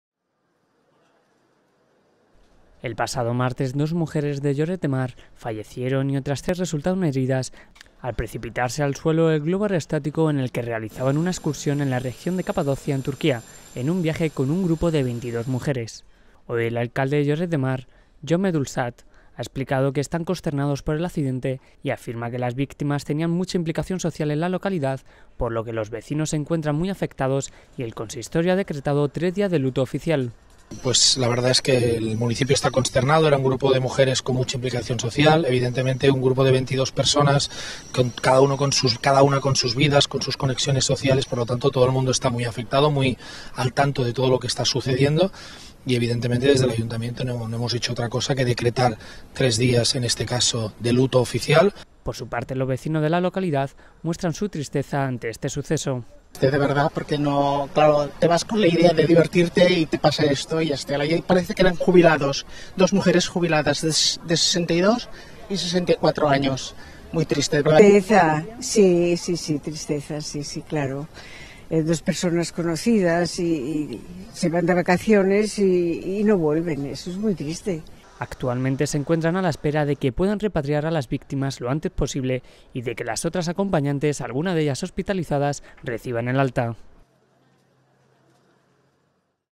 Consternación en Lloret de Mar tras el fallecimiento de dos vecinas en Capadocia
El pasado martes dos mujeres, de Lloret de Mar, fallecieron y otras tres resultaron heridas, al precipitarse al suelo el globo aerostático en el que realizaban una excursión en la región de Capadocia, en Turquía, en un viaje con un grupo de 22 mujeres.